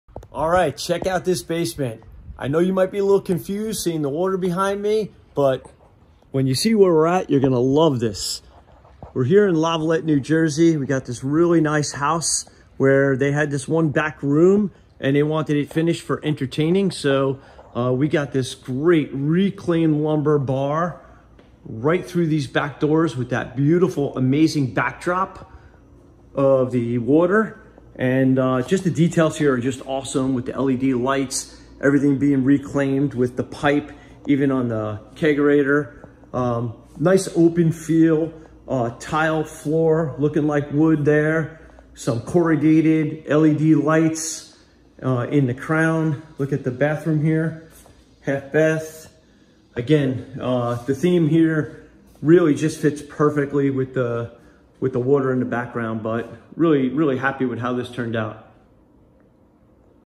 Nautical Theme Basement
This basement in Lavalette New Jersey portrays a nautical theme, located right on the water. The homeowner had a backroom they desired to finish for entertaining purposes. We built them a great reclaimed lumber bar with a beautiful, amazing backdrop, and outstanding detail. The room has a nice open feel with tile which replicates wood, LED lights in the crown molding, corrugated metal shelves, and a half bathroom!

Follow our Instagram - @FinishedBasementNJ
and our Facebook - Finished Basement NJ